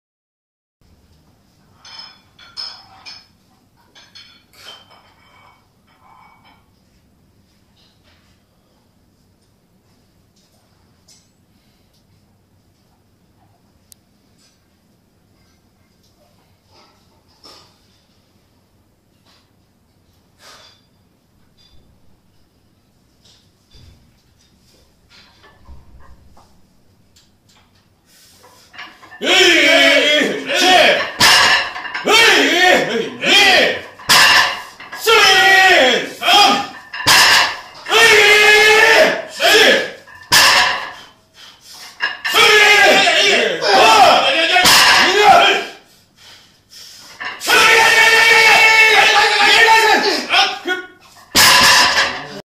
デッドリフト250kg6rep/パワーリフティング/ハリー@93kg
デッドリフトの6レップ自己ベスト更新！
245→250